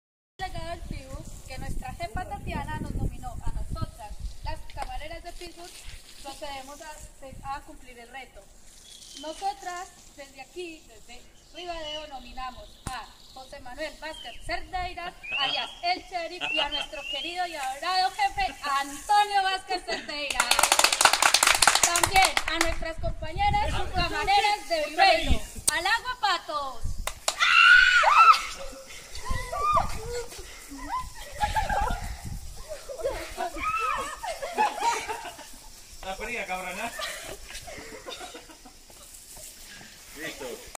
LEGADO DEL TIBU GALIPIZZA RIBADEO
AS CAMAREIRAS DE GALIPIZZA RIBADEO COMEZAN O LEGADO DO TIBU NO ÁMBITO DE GALIPIZZA, E SENDO MOI VALENTES COMEZAN A NOMINAR A:
-O SHERIF LEMAN.
-O XEFE ANTONIO.
-OS COMPAÑEIROS DE GALIPIZZA VIVEIRO.